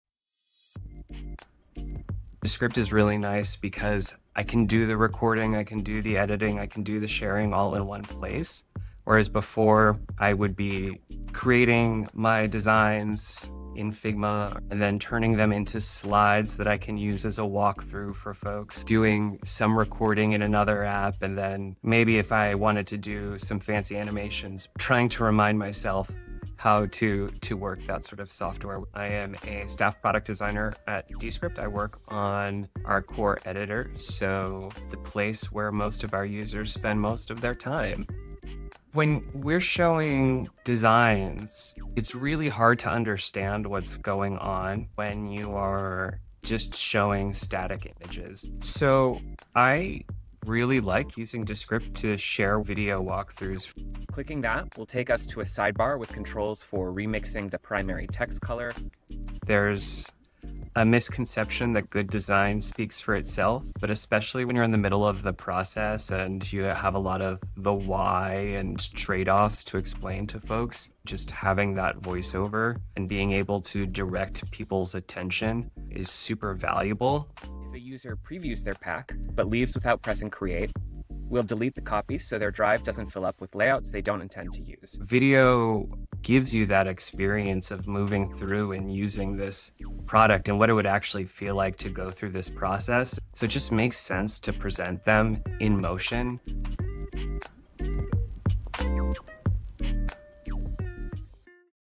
How Designers Use Video To Build Better Products I Descript
See how John Voss, Descript's Head of Product Design, uses video walkthroughs to make design decisions clearer and more collaborative.

Design doesn’t always speak for itself—especially when you’re mid-process. In this behind-the-scenes look, John shares how Descript streamlines his entire workflow—from recording and editing to sharing feedback-ready design walkthroughs in one place.

If you’re still jumping between Figma, slide decks, and screen recorders to explain your design work, this one’s for you.

🎯 In this video, you’ll learn:

✅ Why video beats static designs in the design review process
✅ How video walkthroughs improve clarity and alignment
✅ The role Descript plays in communicating design intent
✅ How product designers can work faster and smarter with fewer tools

🕰 Chapters:
0:00 — One tool for everything: recording, editing, sharing
0:08 — The old workflow (Figma to slides to multiple apps)
0:28 — Meet John, Descript's Head of Product Design
0:38 — The problem with static mockups for product design
0:48 — How Descript makes for better design reviews
0:59 — How video design reviews lead to better communication and feedback